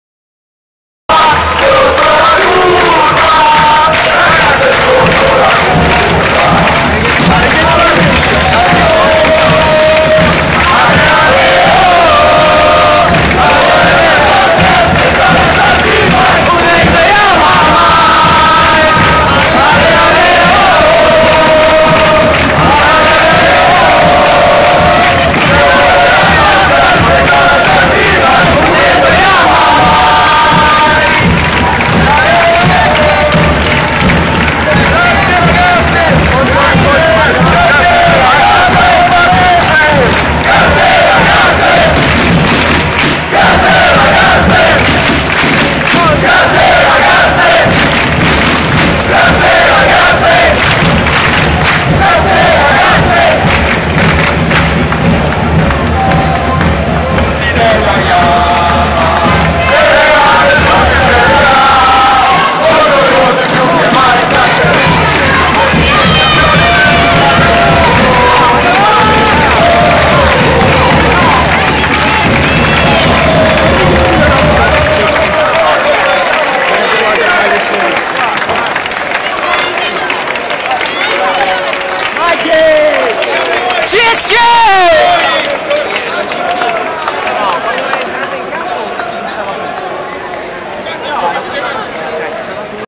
Le Farfalle @ CEV Final Four 2013 Istanbul
UYBA celebrates 3rd place with the fans in Istanbul !!!